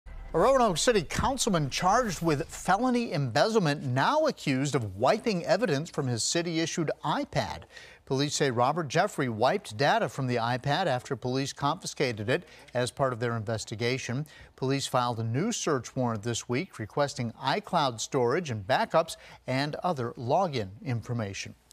Roanoke City councilman charged with felony embezzlement accused of wiping iPad
A Roanoke City Councilman charged with felony embezzlement now accused of wiping evidence from his city-issued iPad.